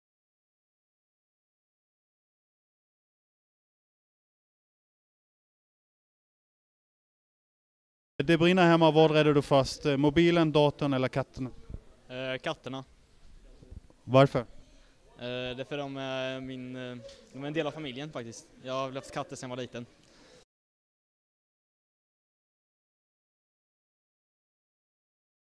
Veckans fråga v 5 år 2011 Östgötatidningen Hilding Jakobsson
Varje vecka ställer vi aktuella, känsliga, fräcka, roliga eller jobbiga frågor till människor i Mjölby med omnejd. Tveka inte att komma fram med era egna frågor alternativt maila oss.